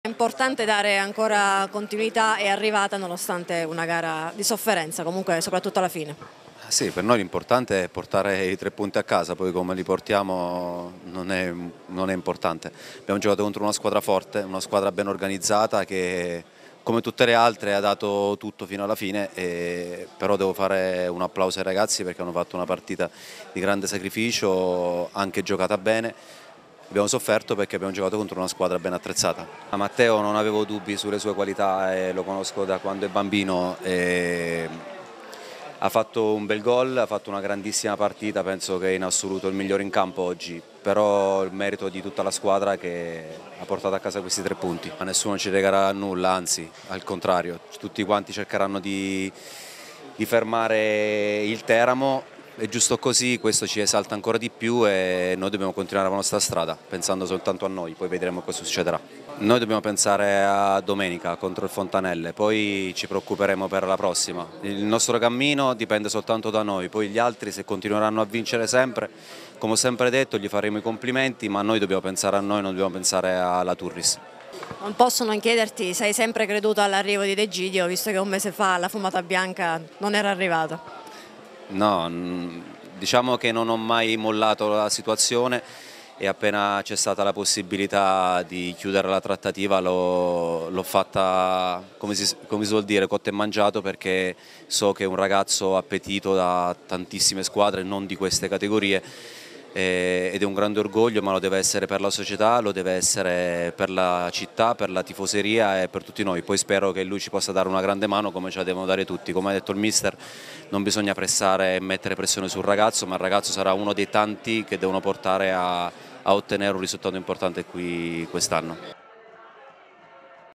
Teramo, D'Ercole: "Con noi tutti danno il massimo, questo ci esalta"
Quella sul Favale è stata la sesta vittoria di fila del Città di Teramo che resta in scia della capolista Turris Val Pescara, prima a punteggio pieno. Il DS Paolo D'Ercole analizza il momento della squadra e torna sulla trattativa che ha riportato in biancorosso Stefano D'Egidio